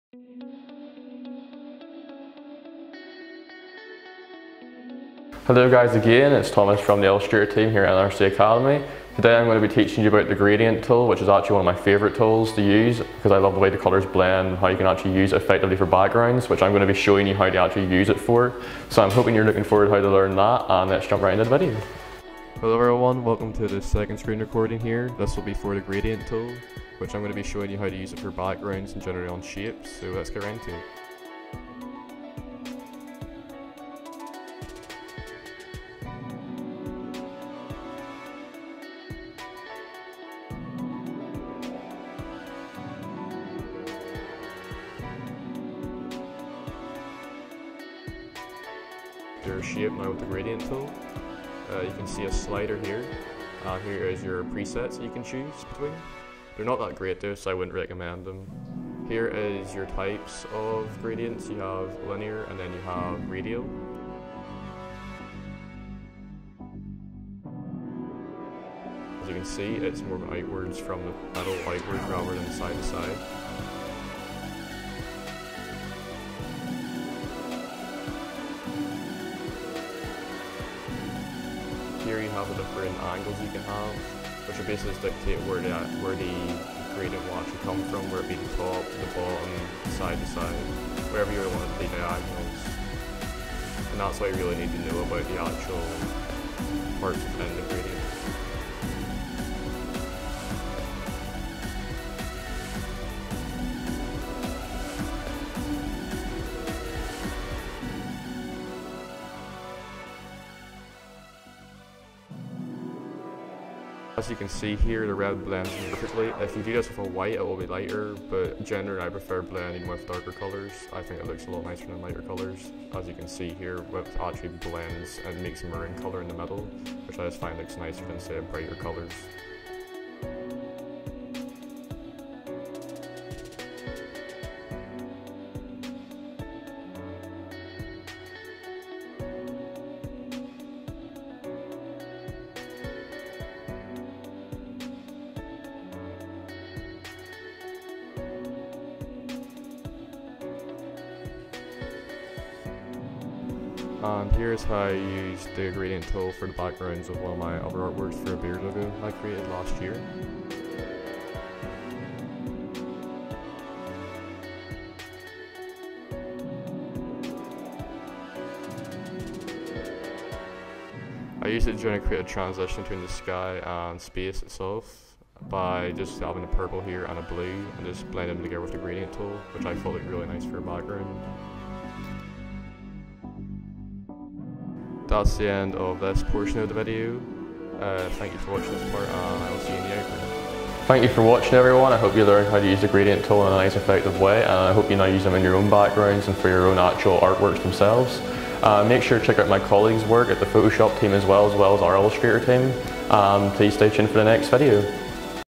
Gradient tool tutorial
Hey again guys this is my 2nd tutorial which was the gradient tool, I hope you enjoyed the video and our looking forward to my last tutorial. make sure to check out our other tutorials from both the illustrator and photoshop teams.